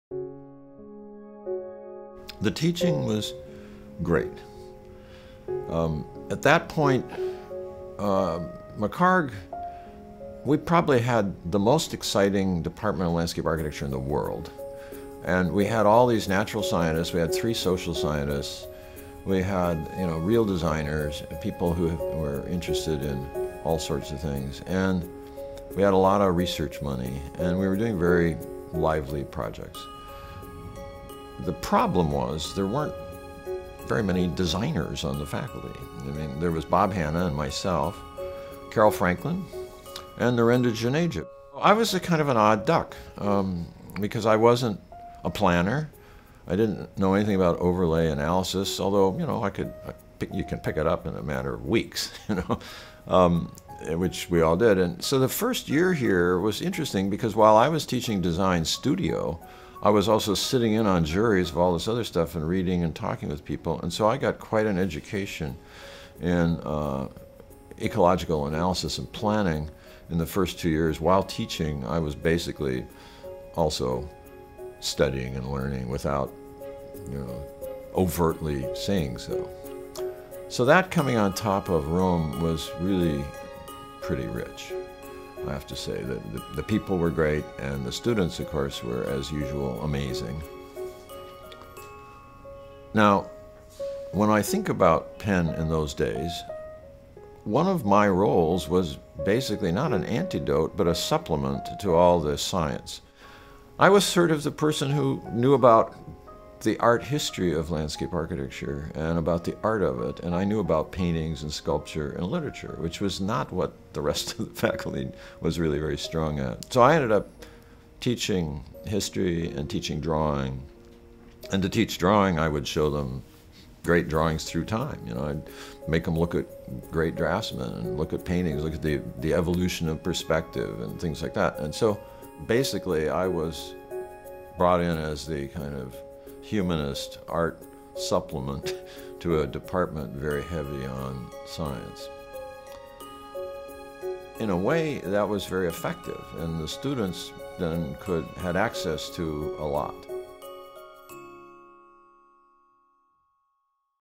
Laurie Olin Biography: Teaching at the University of Pennsylvania [6 of 11]
Hear Laurie Olin explain why Penn's Department of Landscape Architecture was the most exciting in the world.
Interviewed by Charles A. Birnbaum, June 2012.